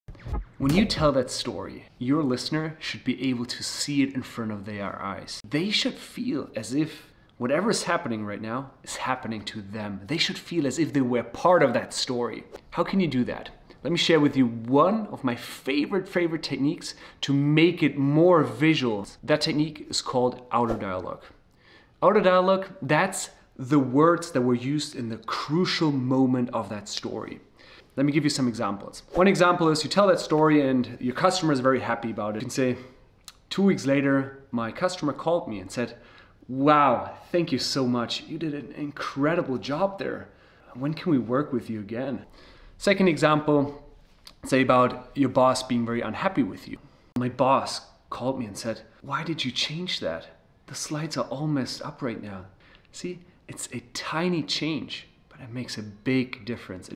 Storytelling Tips - Visual Moments
Goal when you tell a story: Your listeners should see the event in front of their eyes, as if it was happening to them. Check out this short video and find me favourite storytelling technique to make any story more visual!